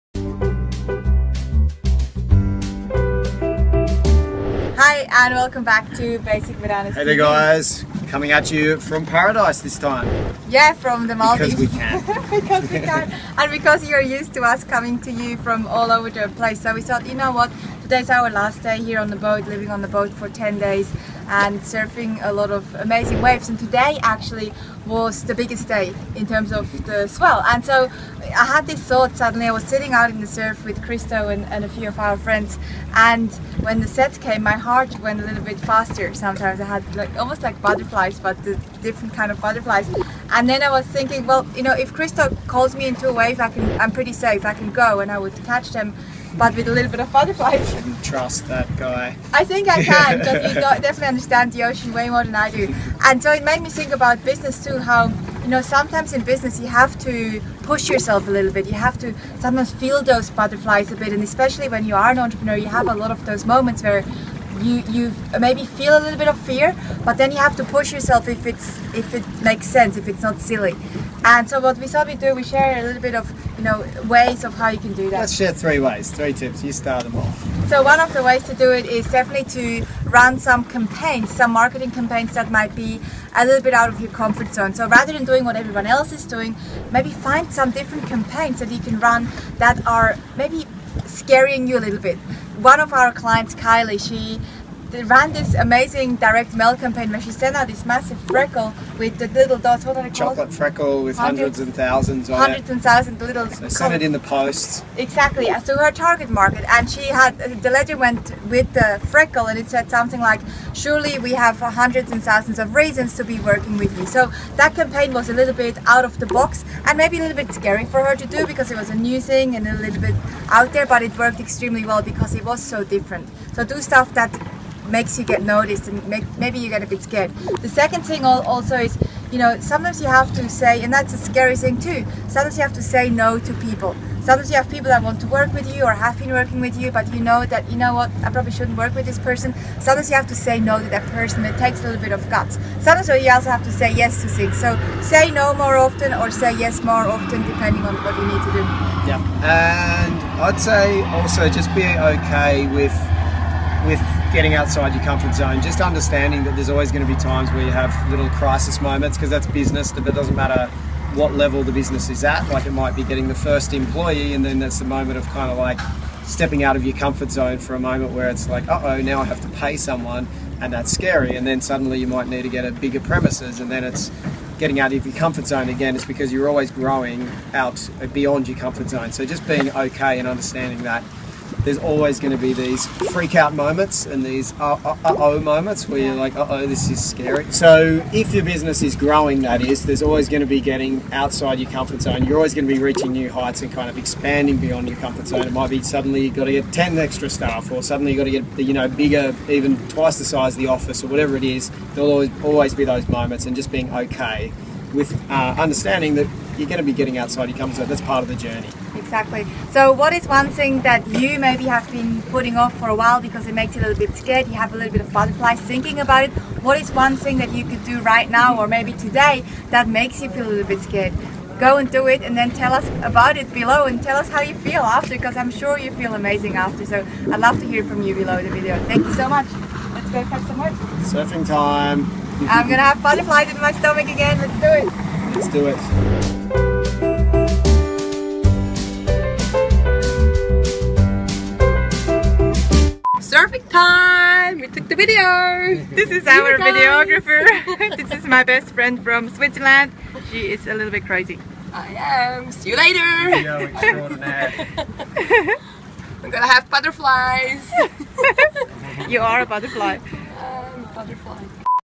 Entrepreneur Video Tip: How to be more courageous
for more valuable marketing tips. We love spoiling our business community!

In this episode of Basic Bananas TV, Franziska and Christo (co-founders of Basic Bananas) share their thoughts on how to be more courageous in your entrepreneurial life.

0:50s Butterflies in the stomach
1:40s The giant freckle campaign
2.30s Saying yes and no more often

What is one thing you could do today that is giving you butterflies but you know would have a positive impact?

As always, thank you for being part of this amazing business tribe! We truly appreciate you!